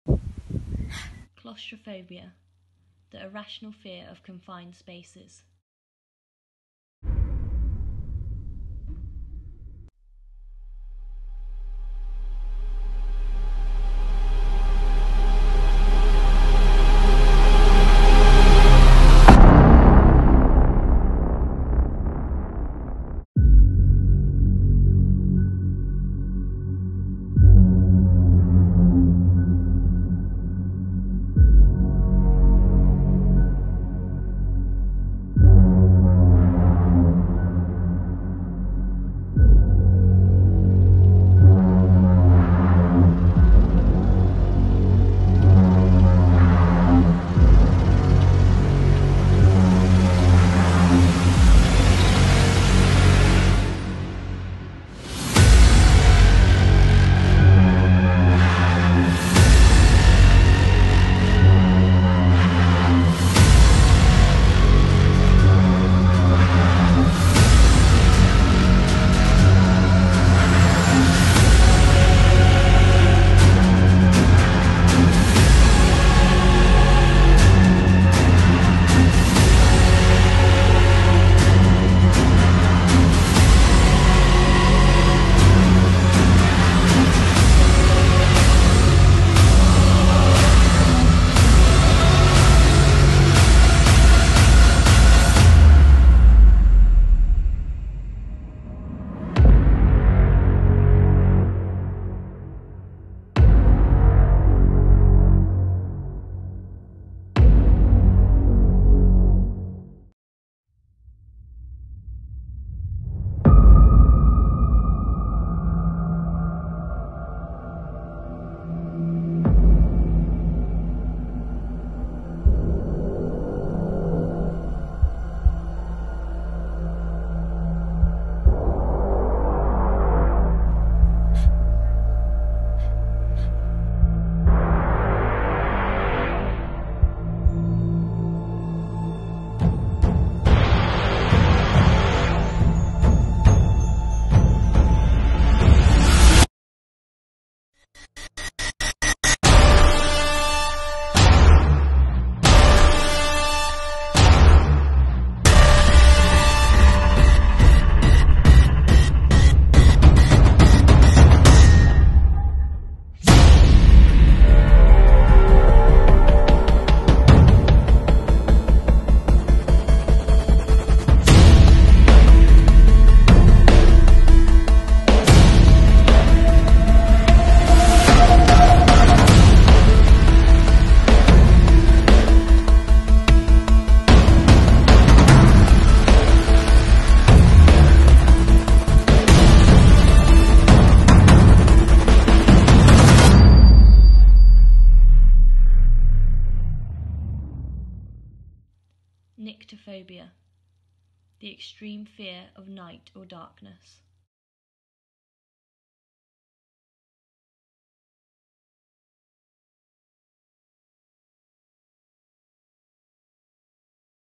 Trapped Ruby Clews Year 9
During lockdown, several GCSE and A-Level Dance students from Coppice Performing Arts School participated in ‘Re-Educate’, a National platform and programme delivered by Flexus Dance Collective to support Dance in Education during such a susceptible time. Over 5 weeks, the young dancers were provided a Flexus Mentor to support their artistic journey as they choreographed, filmed and edited their work.

‘Trapped’ explores phobias and the impact they have on your mind and body. Fear is a natural, powerful and primitive human emotion. The work focuses on Claustrophobia and Nyctophobia; the fear of confined spaces and the fear of the dark.
This work was streamed as part of Flexus Dance Collective’s ‘A Captured Moment’ Digital Dance & Arts Festival in August 2020.